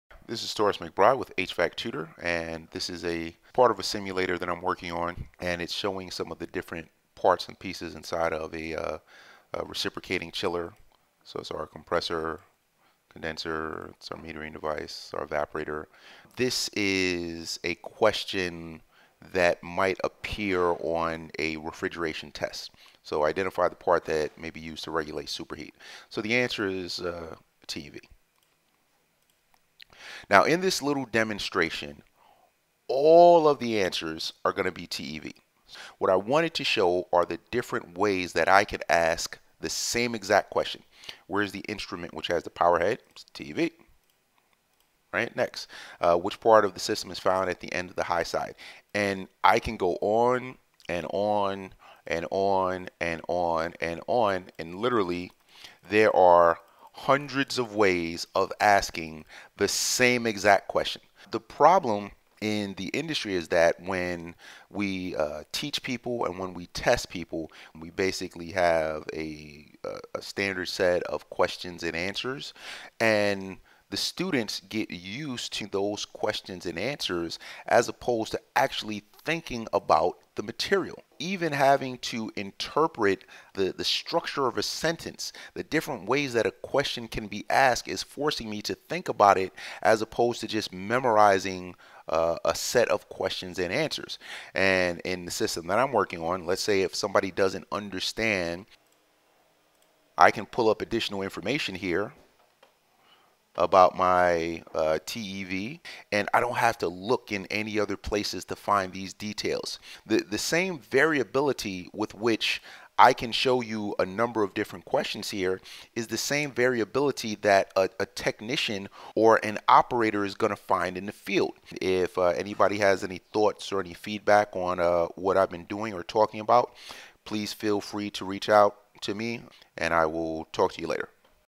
critical thinking
The way we train people today stresses memorization instead of critical thinking. I'm working on a system that will change that.